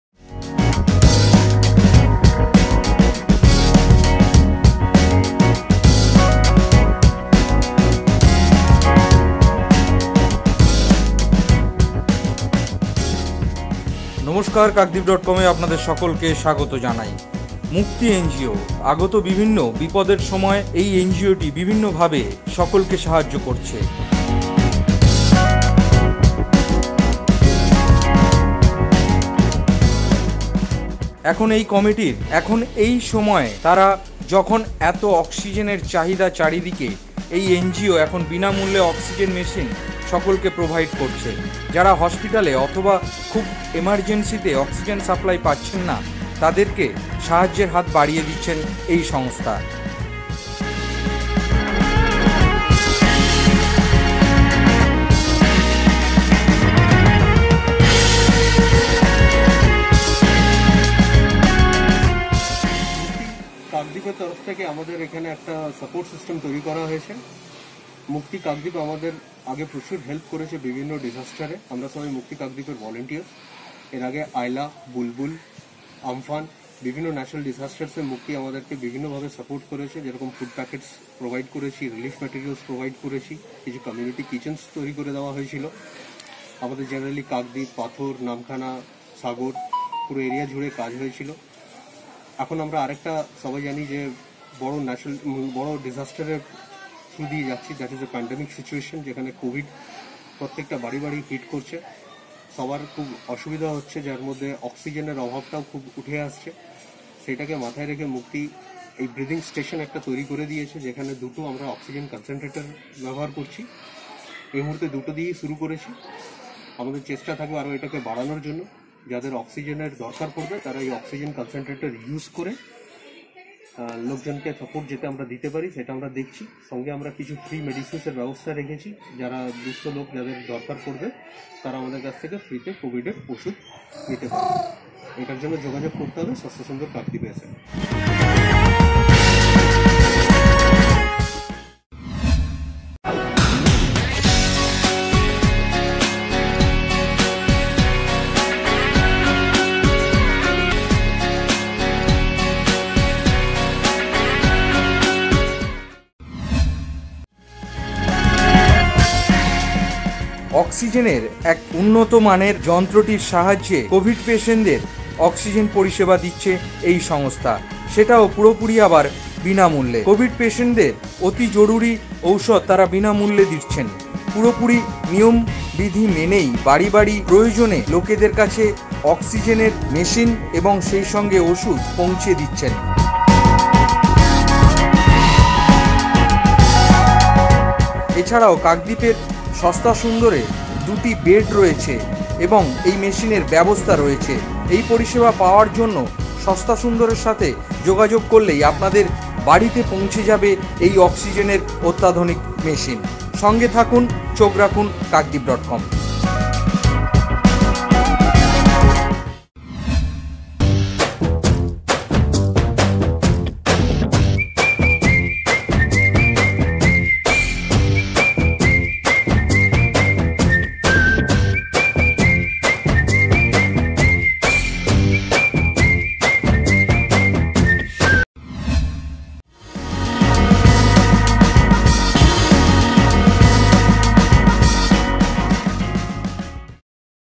বিনামূল্যে অক্সিজেন মেশিন সকলকে প্রোভাইড করছে
মুক্তি এনজিও আগত বিভিন্ন বিপদের সময় সকলকে সাহায্য করেছে। এখন এই কমিটির সময় তারা যখন এত অক্সিজেন এর চাহিদা চারিদিকে এই এনজিও এখন বিনামূল্যে অক্সিজেন মেশিন সকলকে প্রোভাইড করছে যারা হসপিটালে অথবা খুব ইমারজেন্সিতে অক্সিজেন সাপ্লাই পাচ্ছেন না তাদেরকে সাহায্য হাত বাড়িয়ে দিচ্ছে এই সংস্থাটি।
অক্সিজেন কনসেনট্রেটর এই যন্ত্রটি সাহায্যে কোভিদ পেশেন্টদের অক্সিজেনের পরিষেবা দিচ্ছে এই সংস্থাটি সেটাও আবার পুরোপুরি বিনামূল্য তার সাথে কোভিদ পেশেন্টদের অতি জরুরী ঔষধ তারা বিনামূল্যে দিচ্ছে।
পুরোপুরি নিয়ম বিধি মেনে বাড়ি বাড়ি প্রয়োজনে লোকেদের কাছে এই অক্সিজেন কনসেনট্রেটর পৌঁছে দিচ্ছে। এছাড়াও কাকদ্বীপ সস্তা সুন্দর দুটো বেড আর এই মেশিনের ব্যবস্থা রাখা হয়েছে।
এই পরিষেবা পাওয়ার জন্য সস্তা সুন্দর সাথে যোগাযোগ করলেই আপনার বাড়ি পৌঁছে যাবে এই কনসেনট্রেটেড বিনামূল্যে